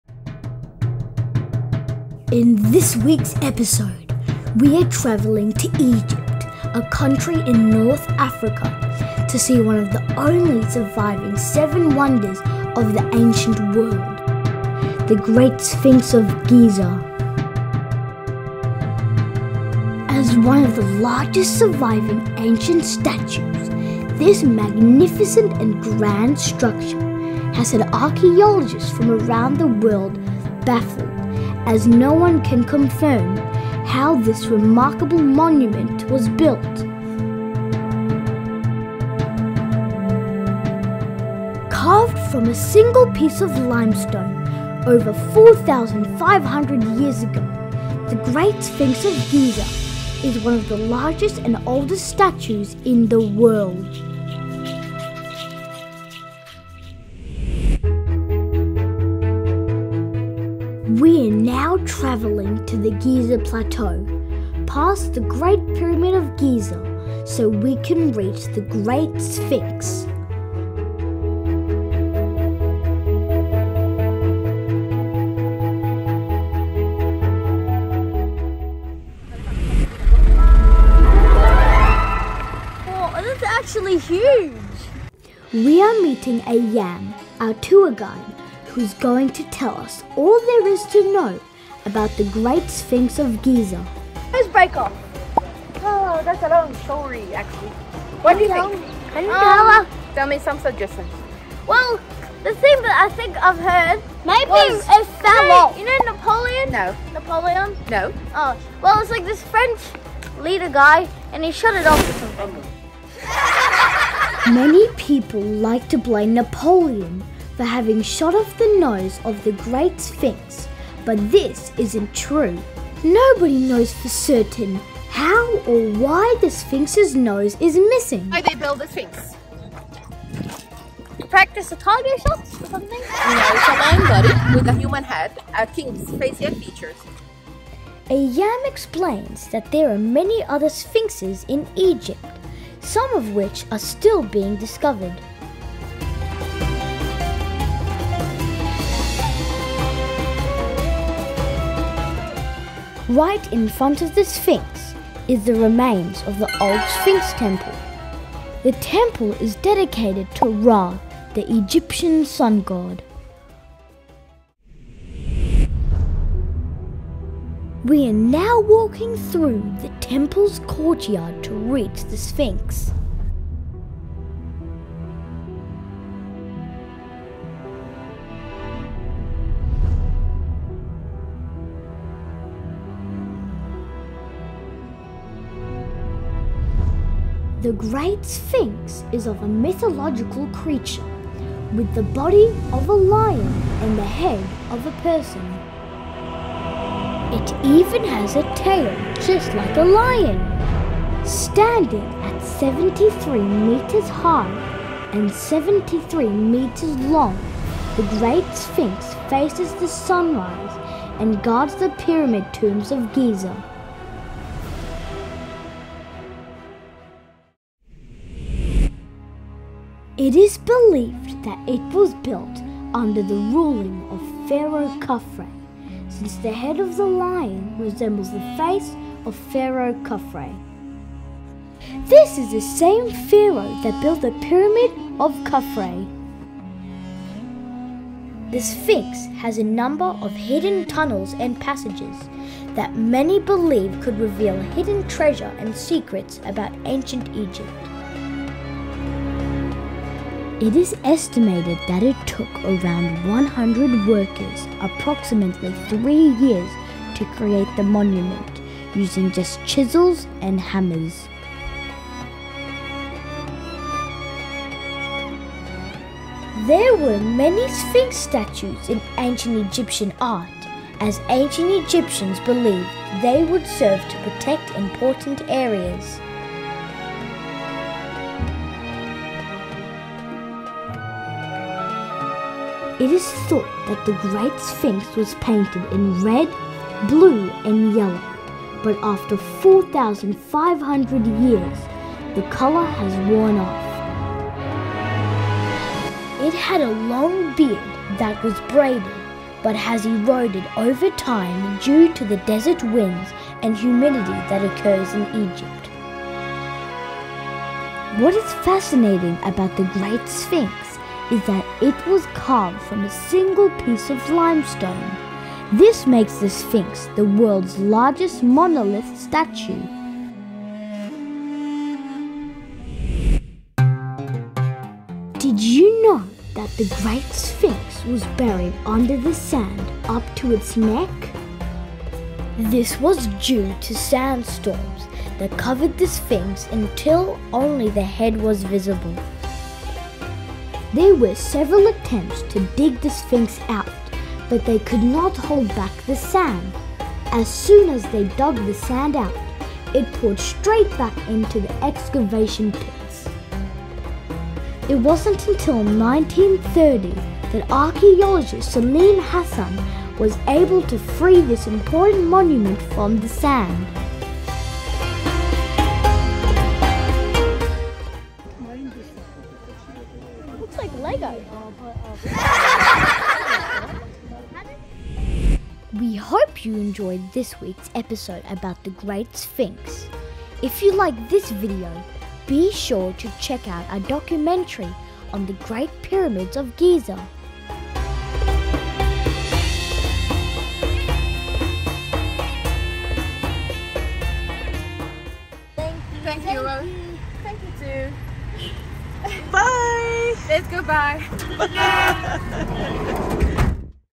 The Great Sphinx of Giza - All You Need To Know! Ancient Egypt for Kids | History for Kids
Oliver and Lucas want to learn about Ancient Egypt and the Great Sphinx of Giza! So they decide to travel to Egypt, to see one of the only surviving Seven Wonders of the Ancient World, The Great Sphinx of Giza.

They travel to the Great Pyramid of Giza to meet Ayam, who will take them on a tour of the Giza Complex where she will share facts about the Sphinx for kids and Ancient Egyptian History for Kids.

Why was the Sphinx Built? Who built the Great Sphinx? Why was the nose of the Sphinx cut off? All these questions will be answered in this fun Ancient History for Kids episode.

As one of the largest surviving ancient statues, this magnificent and grand structure has had archeologists from around the world baffled as no one can confirm how this remarkable monument was built!
Carved from a single piece of limestone over 4500 year ago, The Great Sphinx of Giza is one of the largest and oldest statues in the world.

The Great Sphinx, is of a mythological creature with the body of a lion and the head of a person. It even has a tail just like a lion.

The Great Sphinx was carved from a single piece of limestone! This makes the Sphinx the world’s largest monolith statue.

We hope you enjoyed this week’s episode about the Great Sphinx.
If you liked this video be sure to check out our documentary on the Great Pyramids of Giza 👉🏽

Acknowledgments:
Thank you Ayam our tour guide who was able to answer the 100 question thrown at her from Oliver and Lucas about the The Great Pyramids! She can be found on instagram at 👉🏽